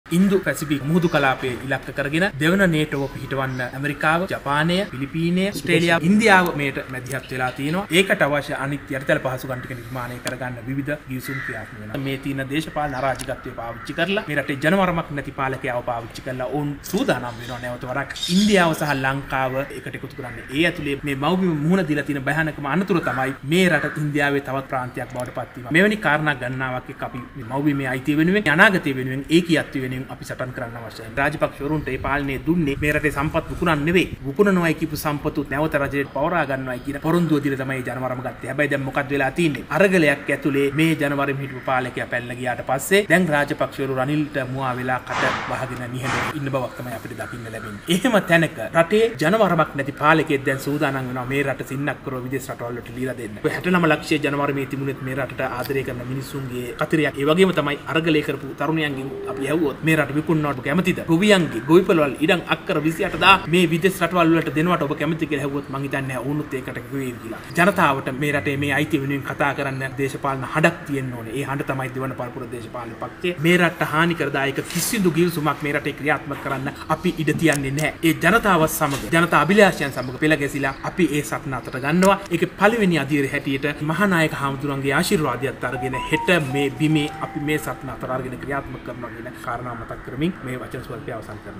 ඉන්දියානූ ව්‍යාප්තවාදයට එරෙහි සටනට පණ දෙන්න අපි සූදානම් - 2024.03.19
උවිඳු විජේවීර මහතා මාධ්‍ය ඉදිරියේ දැක්වූ අදහස්